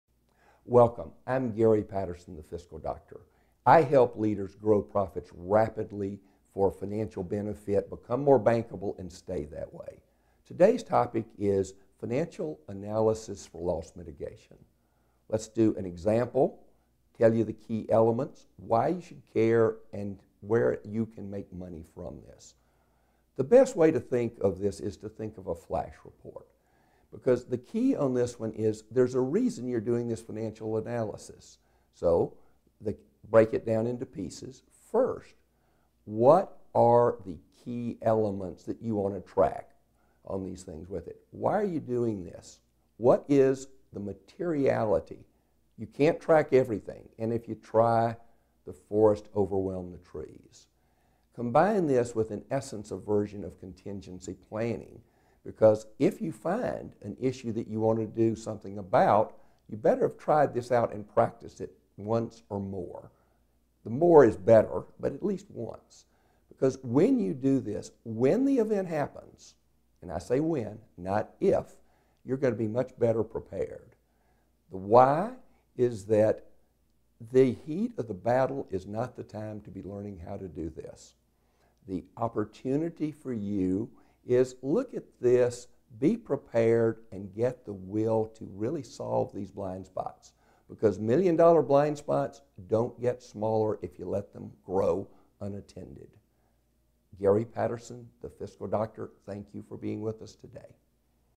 Financial Analysis for Loss Mitigation
Financial Analysis for Loss Mitigation. Part of the series: Increasing Profitability. Loss mitigation and financial analysis is a little bit like managing something called a flash report. Find out about financial analysis for loss mitigation with help from the fiscal doctor in this free video clip.